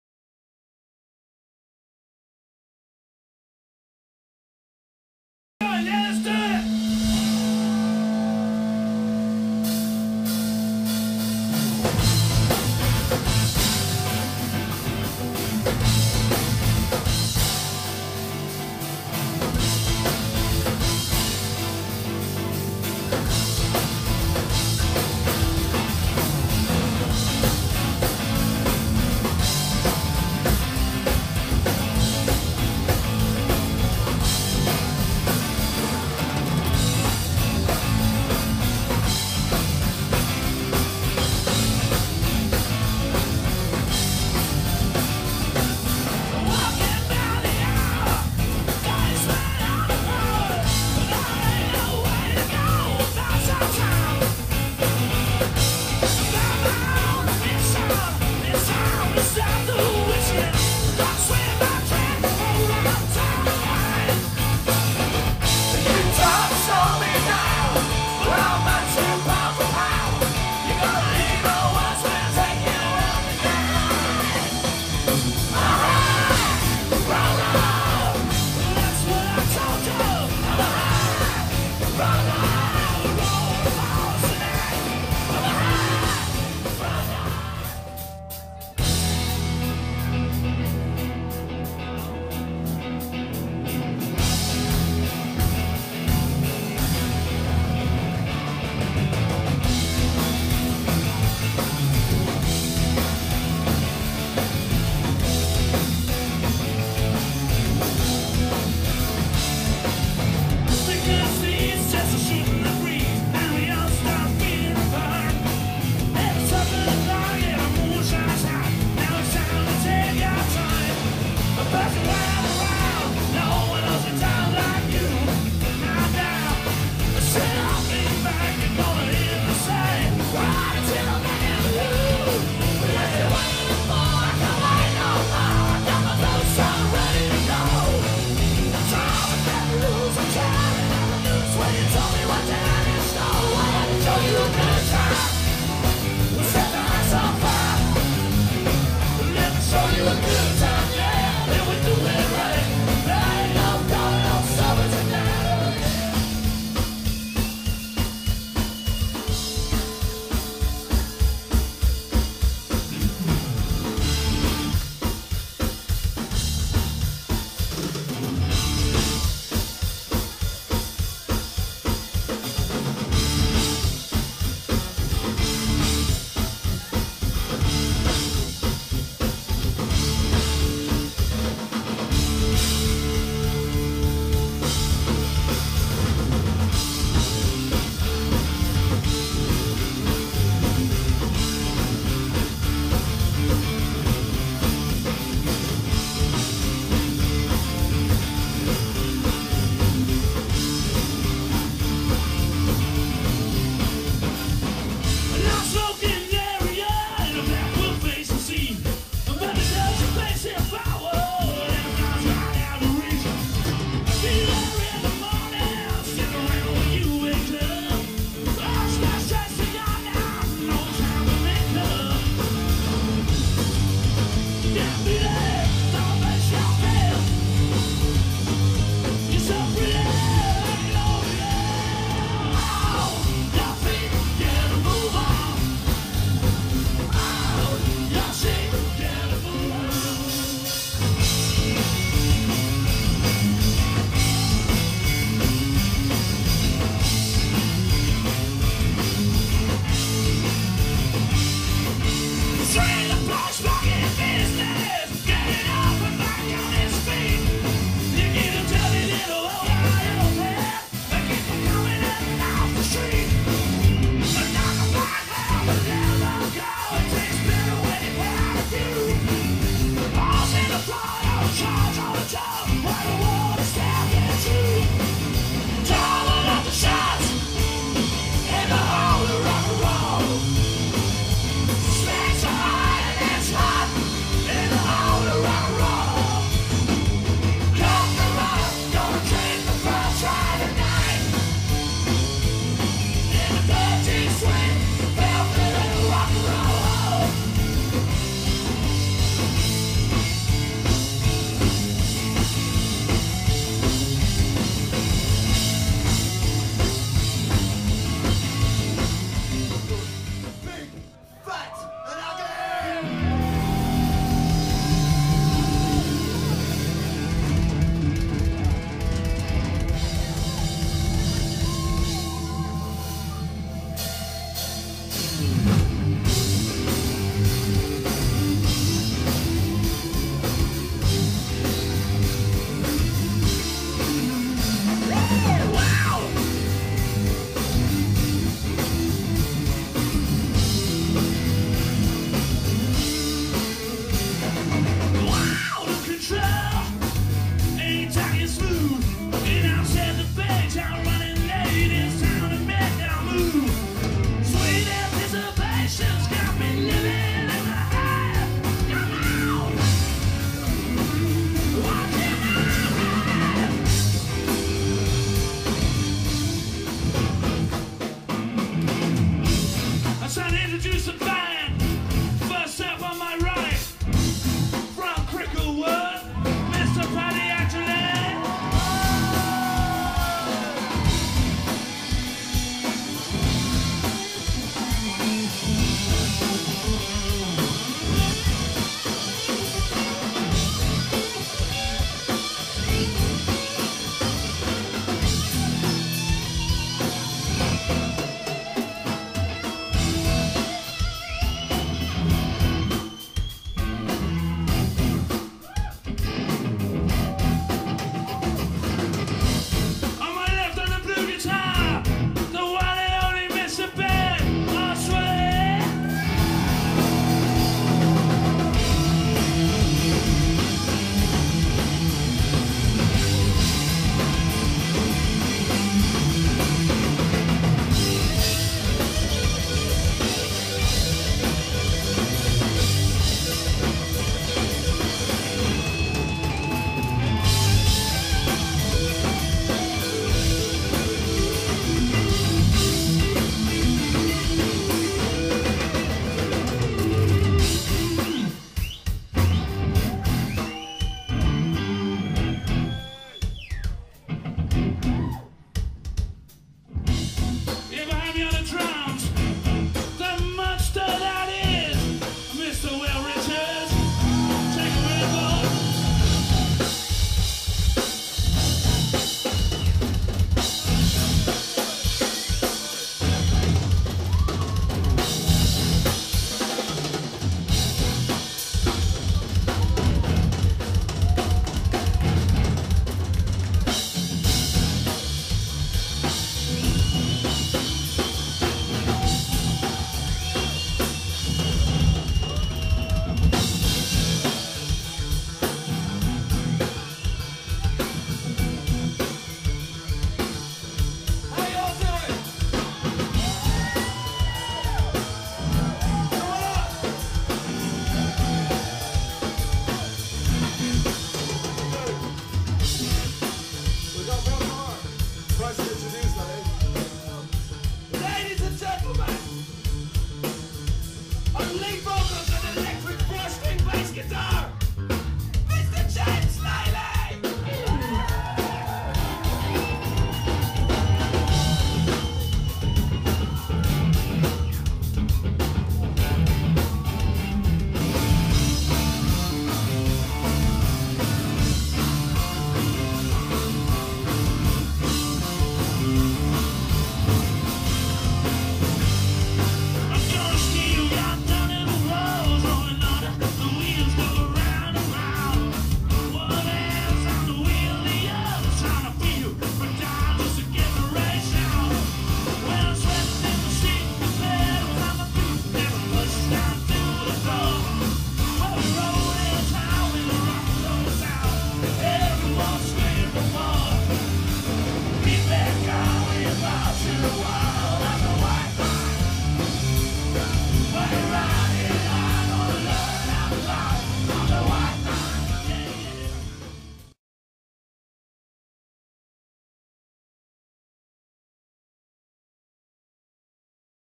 four wheel drive at the monto water rats.wmv
A 10 minute video of highlights of 'Four Wheel Drives' gig at the Monto Water Rats,London.Filmed with a zoom q3hd pocket camera.